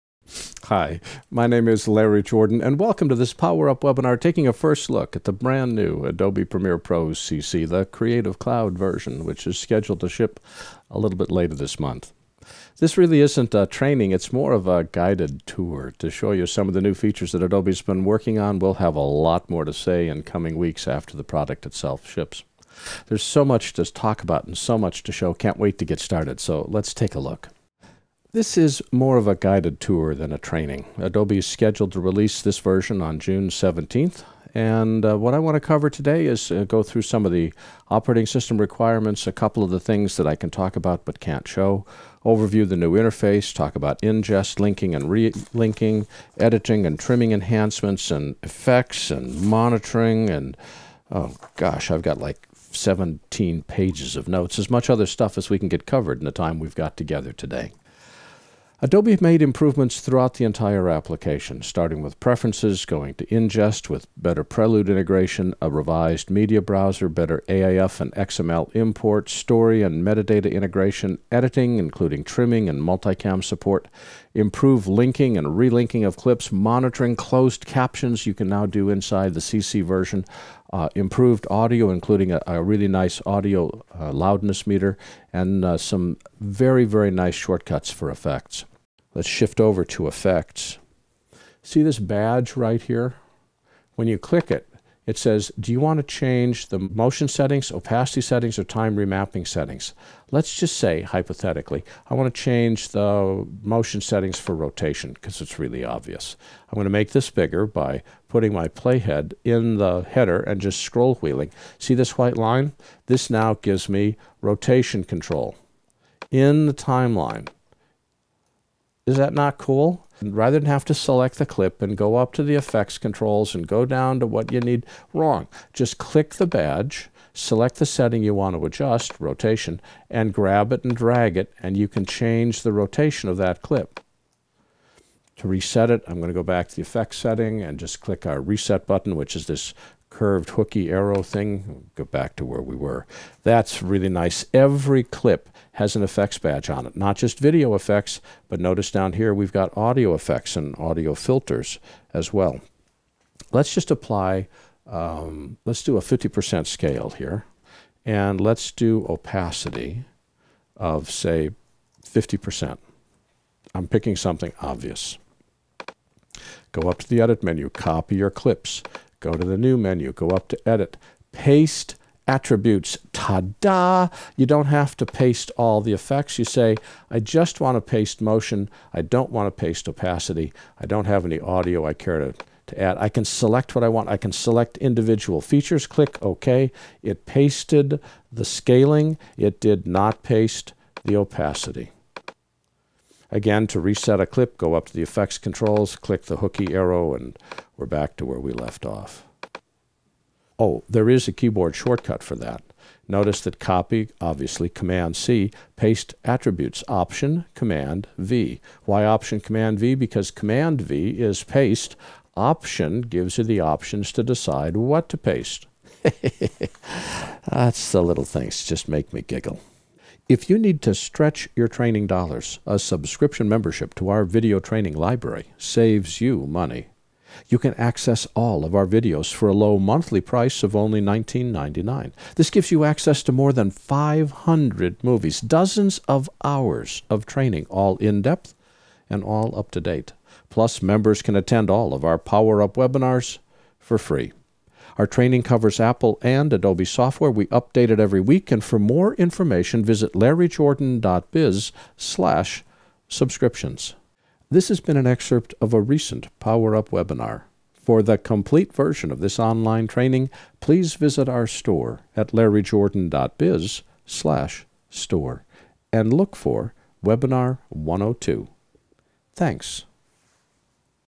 Adobe Premiere Pro CC: First Look (Webinar Preview)
- Anxious to discover all of the updates and improvements in Adobe Premiere Pro CC? You're in good company! This preview of our "Adobe Premiere Pro CC: First Look" webinar gives you a quick overview. For more, check out our full "Adobe Premiere Pro CC: First Look" webinar!